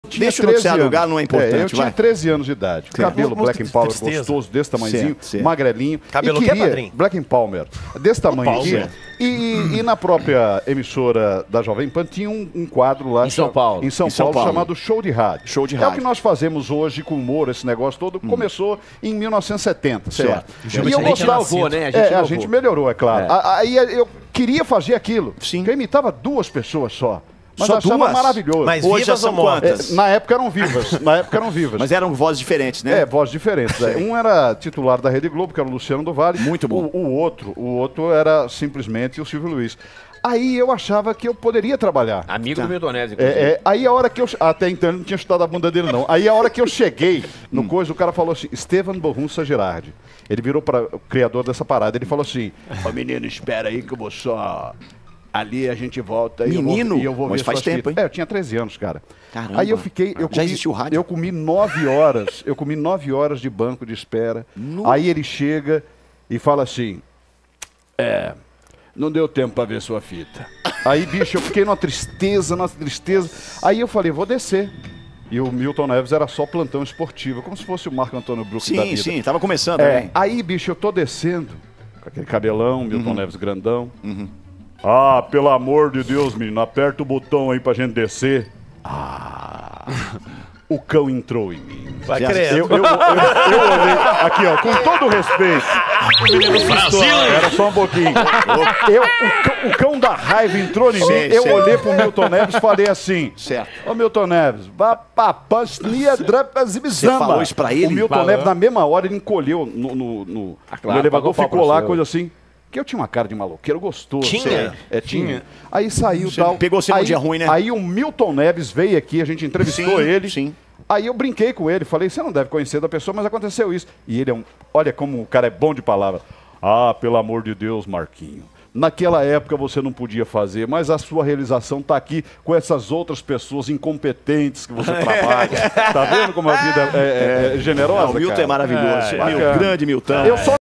Padreco XINGOU o MILTON NEVES
Todo mundo sabe que o Padreco ama uma história né? E na bagagem dele são muitas para contar, viu!

Hoje dia de contar sobre a primeira vez que ele esteve com Jornalista e apresentador Milton Neves. Confere ai.